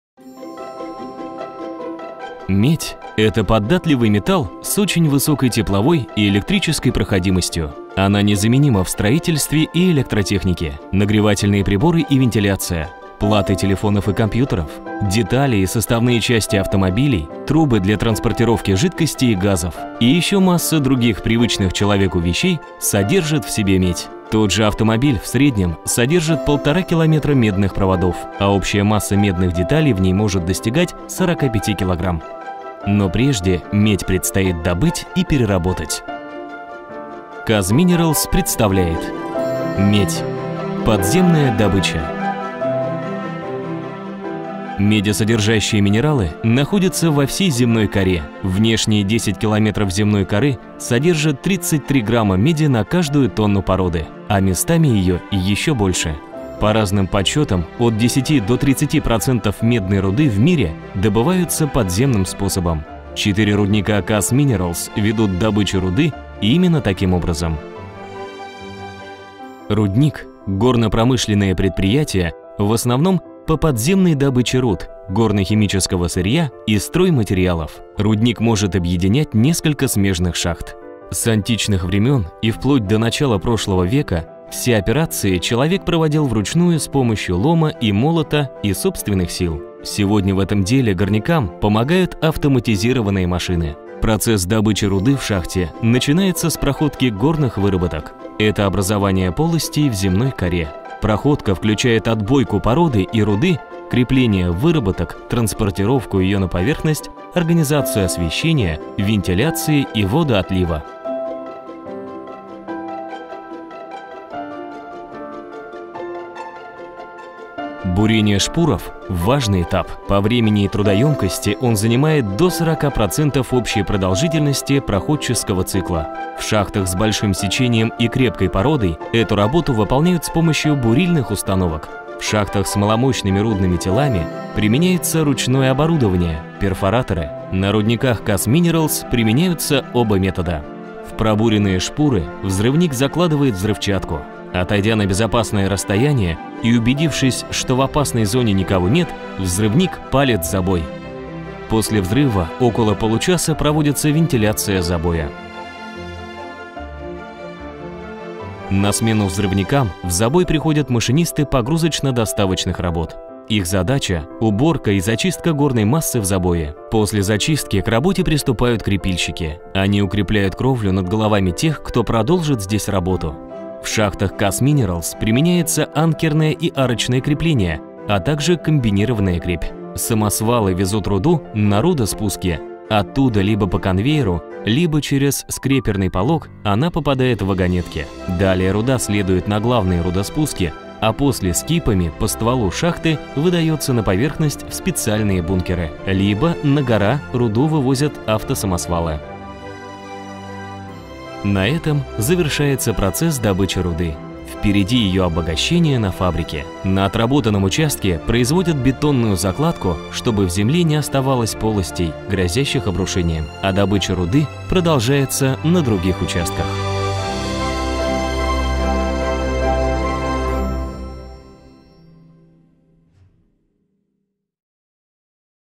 Медь. Подземная добыча
Медь - это податливый металл с очень высокой тепловой и электрической проходимостью.  Она незаменима в строительстве и электротехнике. Нагревательные приборы и вентиляция, платы телефонов и компьютеров. Детали и составные части автомобилей, трубы для транспортировки жидкостей и газов. И еще масса других привычных человеку вещей содержат в себе медь.

KAZ Minerals представляет